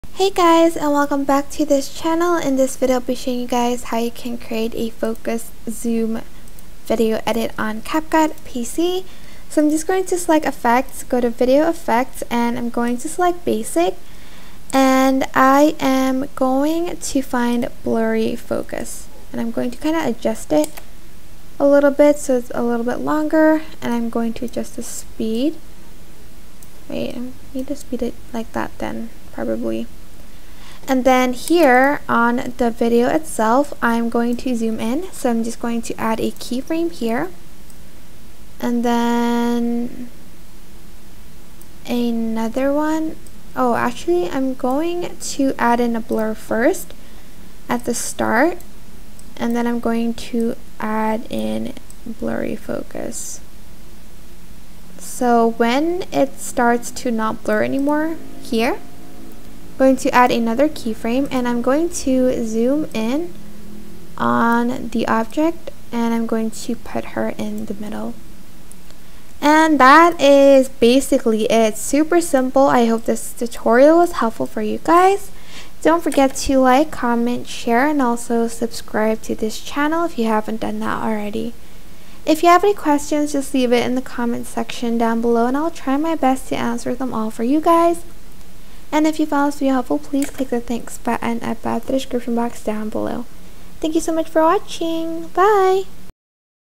How To Focus Zoom In CapCut PC
Want to focus zoom in CapCut PC? Here's how you can do that.